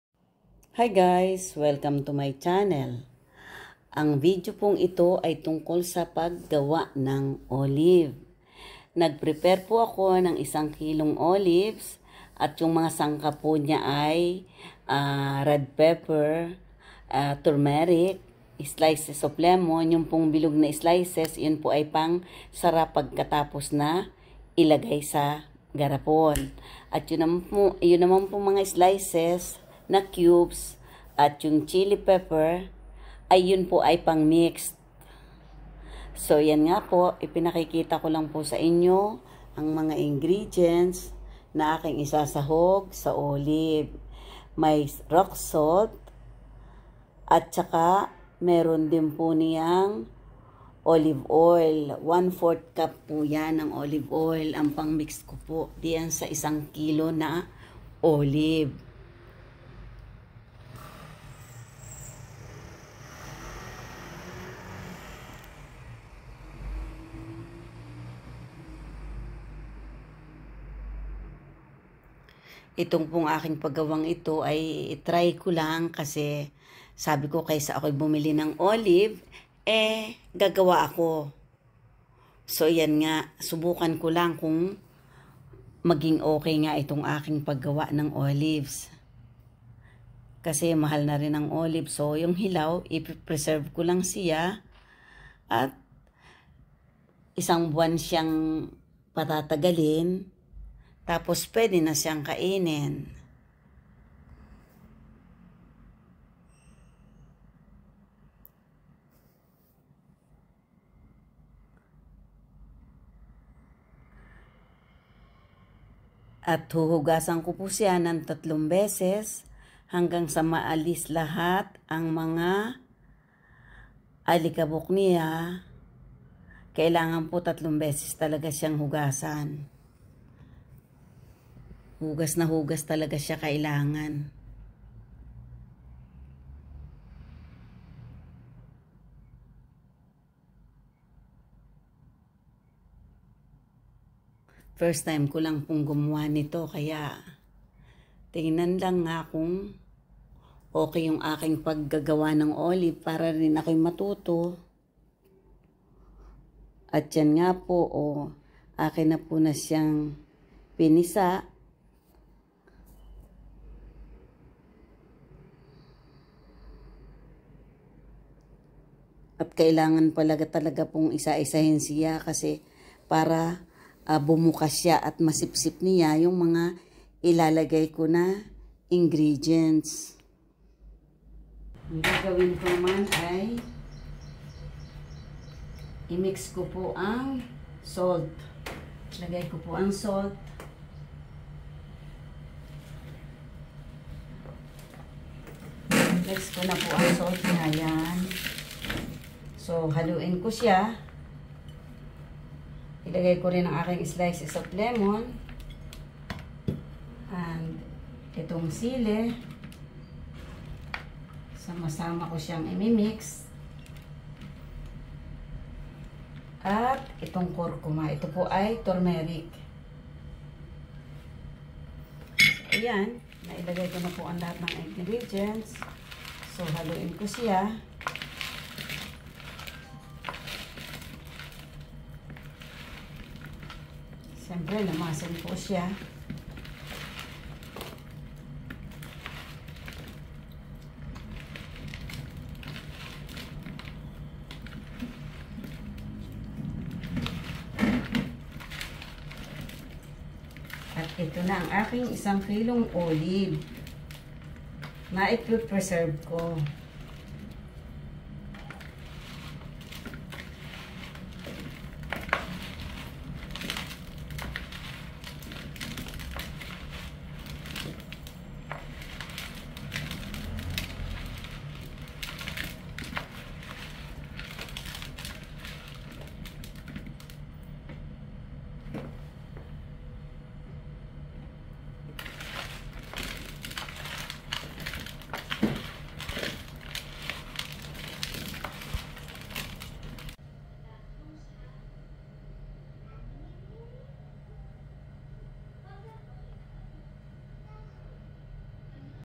howto make home made olives 🫒
I try to make my own homemade olives 🫒